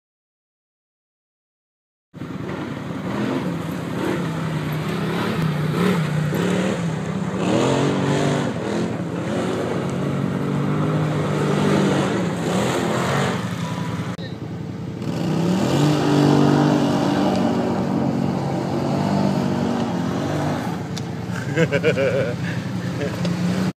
John Deere RSX850i Action - UTV Off-Road Magazine
A quick spin in the 2013 John Deere RSX850i